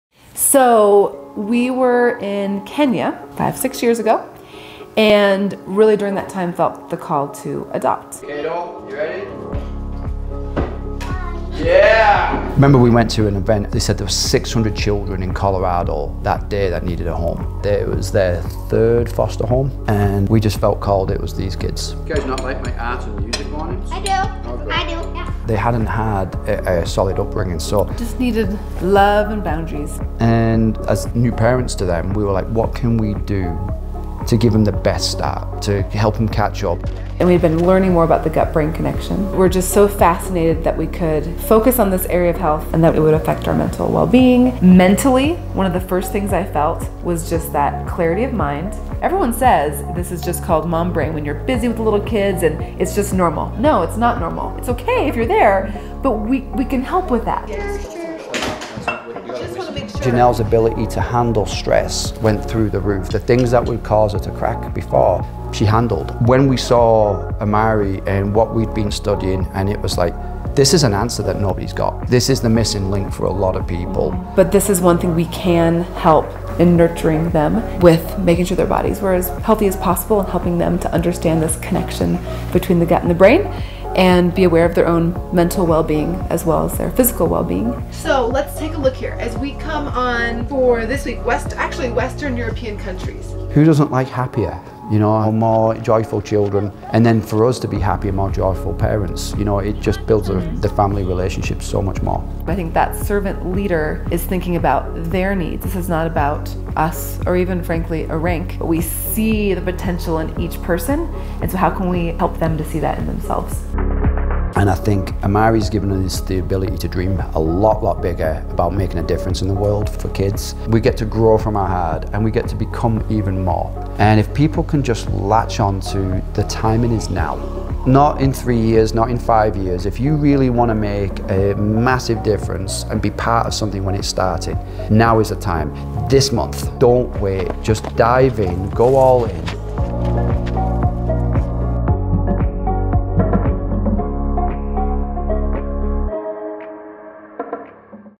Amare Global Servant Leaders | The Devlin family
This is the Devlin family. Get to know what a day in the life of the Devlins is like - homemade pancakes, homeschooling, and an adventure in the Colorado outdoors! The Devlins have such a contagious energy and a passion for helping children.

Client: Amare Global
Director/Producer: Leah de Leon
DP & Editor: Matt Meredith
Production Company: Unreel Stories